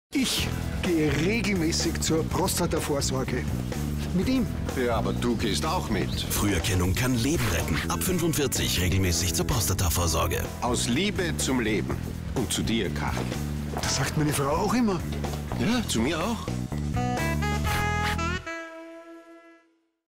Loose Tie 2015 | TV-Spot Hohenlohe & Trenkwitz
Auftraggeber:
Österreichische Krebshilfe gemeinsam mit der Österreichischen Gesellschaft für Urologie und dem Berufsverband der Urologen

Konzeption:
Die Tafelrunde (Gerald Fleischhacker & Klaus Oppitz) gemeinsam mit der Österreichischen Krebshilfe

Darsteller (alle ehrenamtlich):
Karl Hohenlohe & Christoph Wagner-Trenkwitz

Produktion:
Seven Film und Postproduktion GmbH, Stefan Pfundner

Ton: Tonstudio HOLLY
Sprecher: Armin Rogl
Grafik: Ralf Ricker